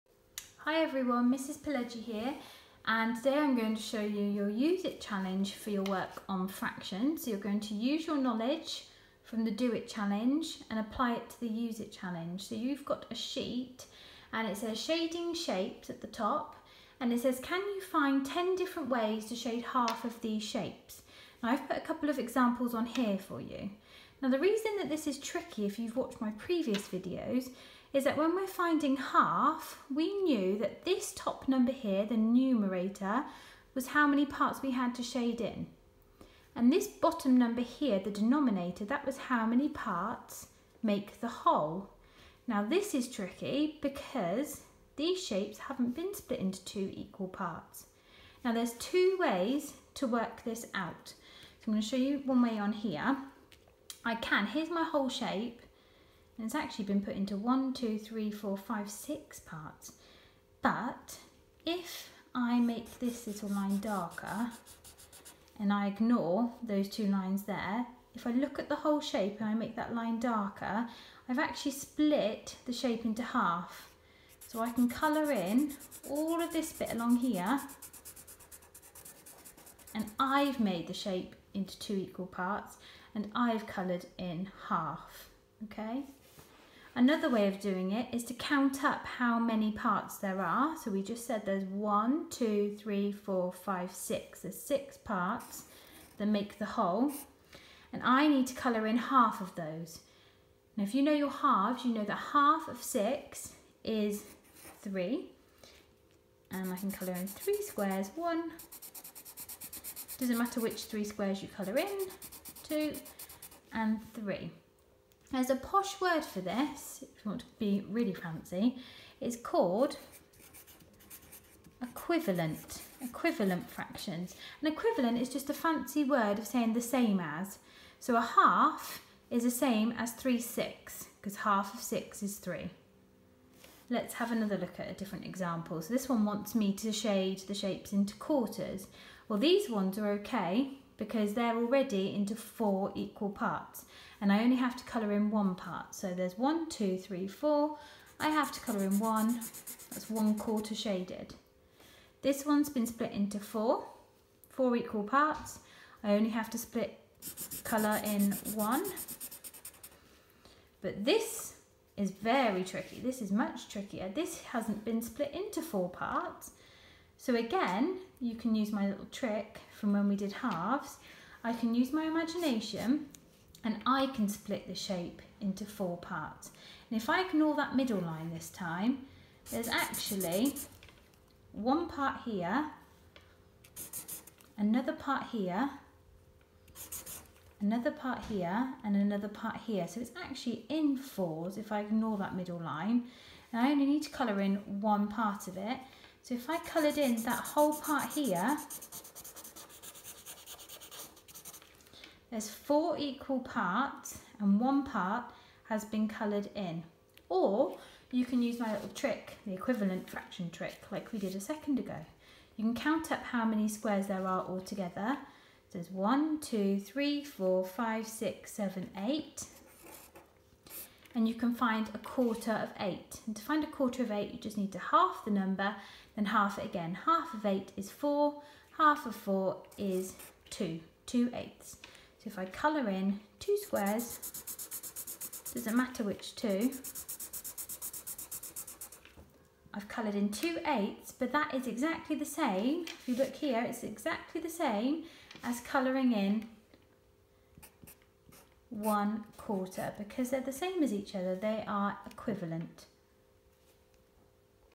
Year 2: Maths - Fractions USE IT
In this video Mrs Pileggi models how to complete the USE IT activity.